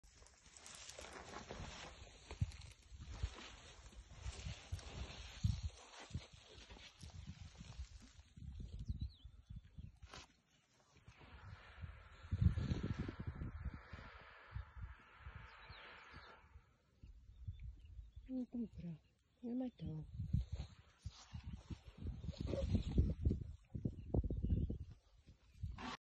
Skin Care For Elephants ☀️
Kumbura takes a sand shower out in the reserve, making the most of loose sand that she digs up with her sturdy feet.

An African elephant’s skin is its largest organ and can weigh up to 900 kilograms. Covering this skin with mud and sand helps protect elephants when exposed to the hot and merciless African sun from the Equator to the Namib and Sahara deserts, often without a shady tree in sight.

Kumbura learnt, as Khanyisa is learning now, to master both mud bathing and dust showering to protect her from biting insects and the elements of nature.

It is an age-old ritual that helps elephants to survive in their natural habitats.